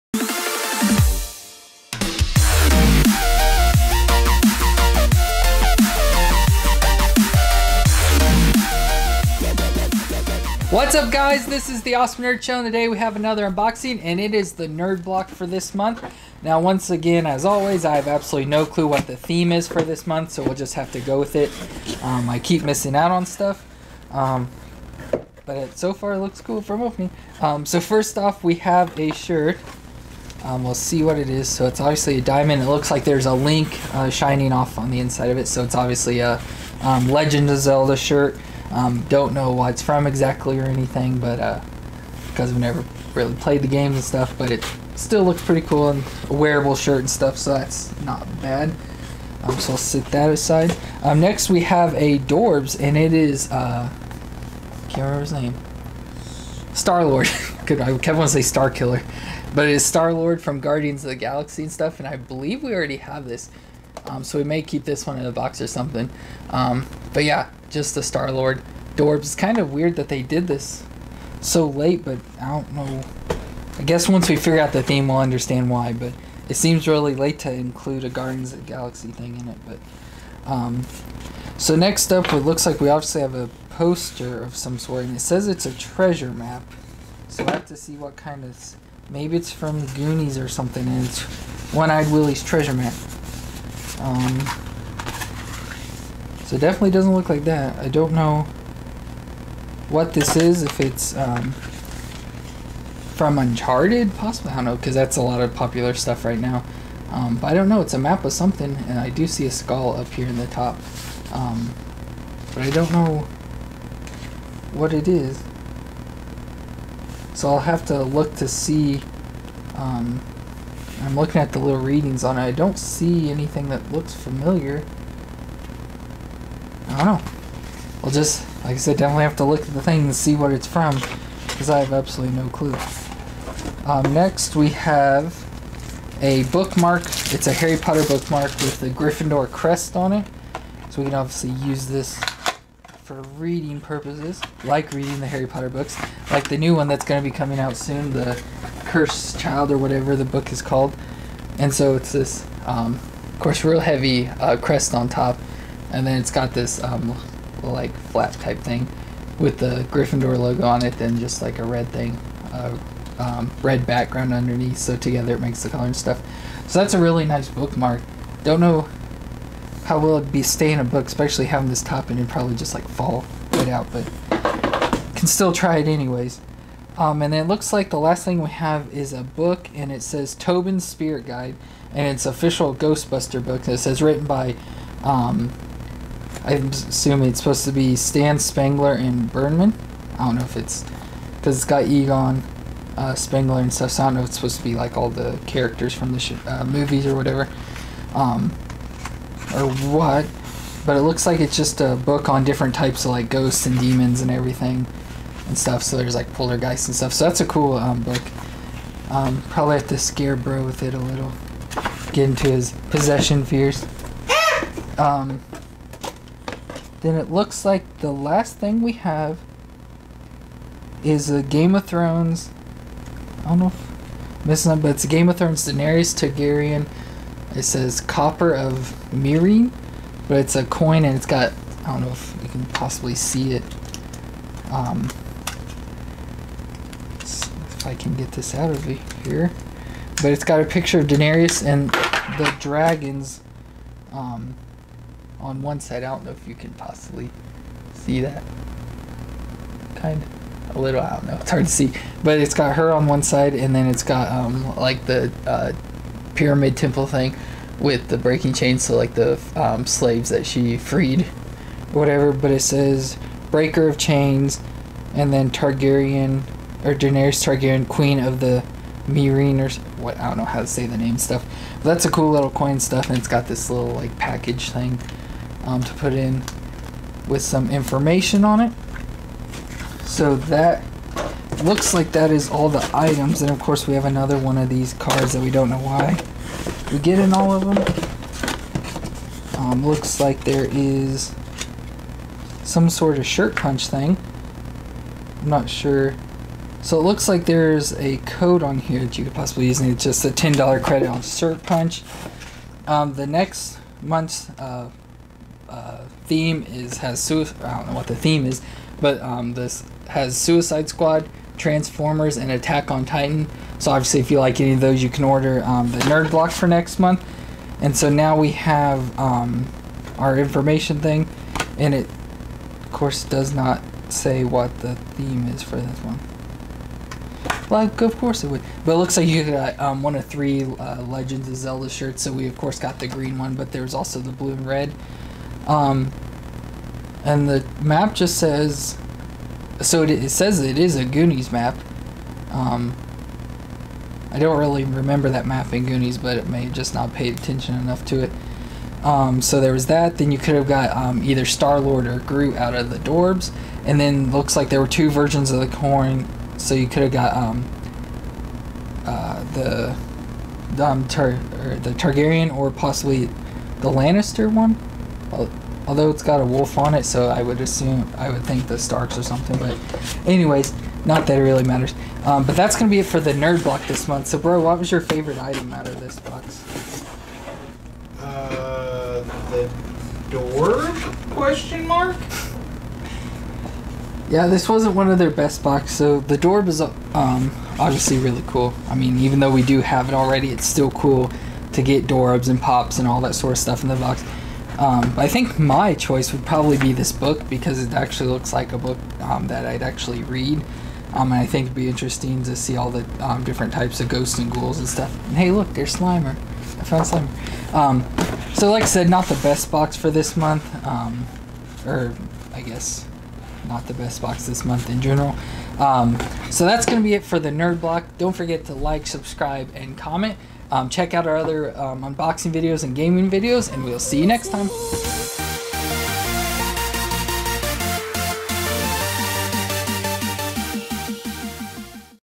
Nerd Block June 2016
Doing a horrible unboxing of this month's Nerd Block of whatever theme you can make up.